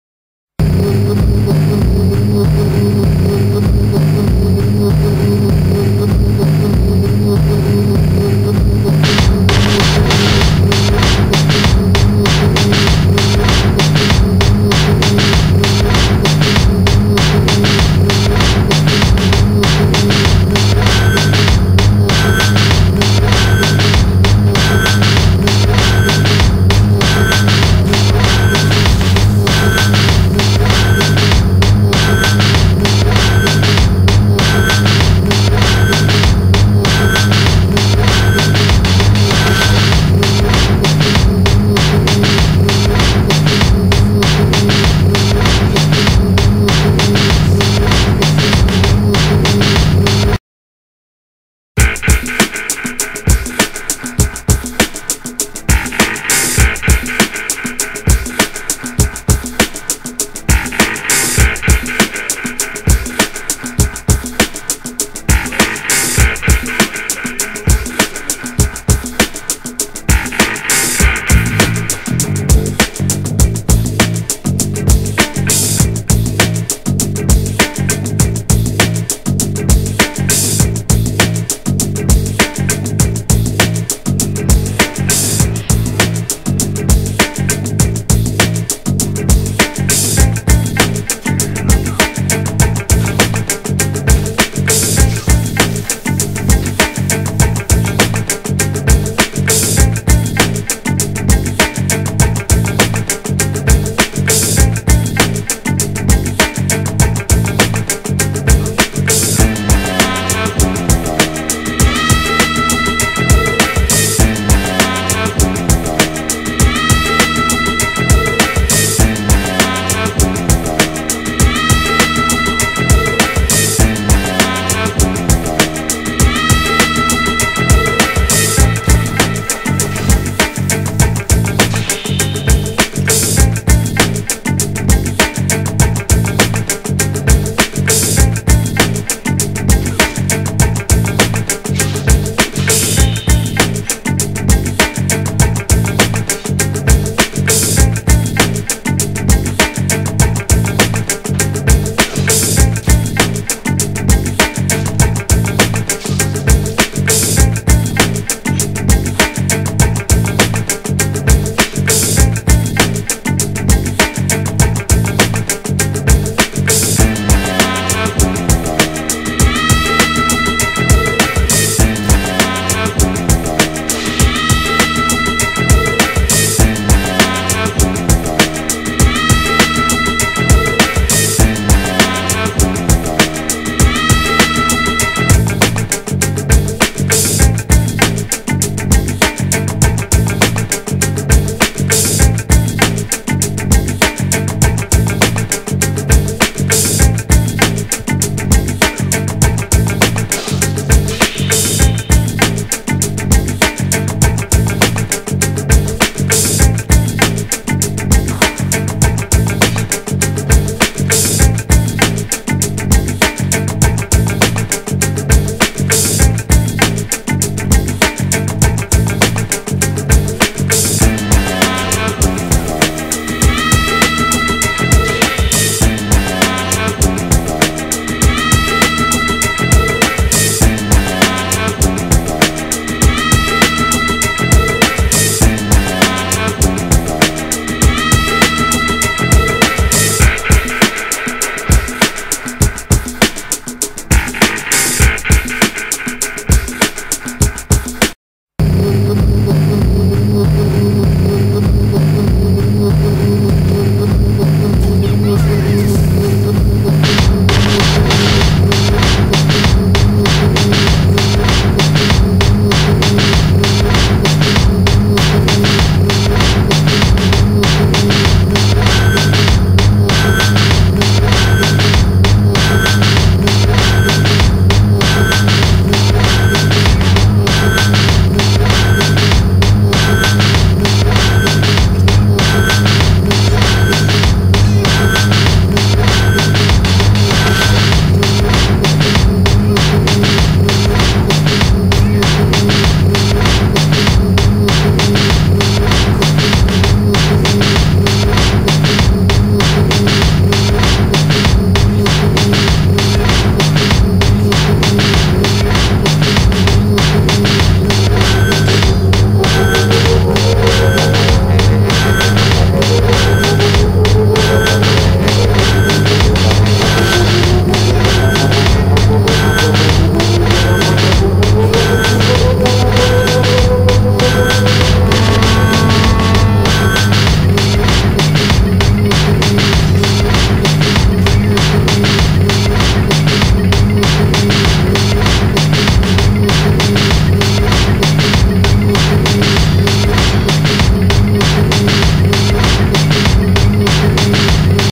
"Lethal Lunacy"
This video shows off a few stealth kills and cutsenes of mission 17. enjoy.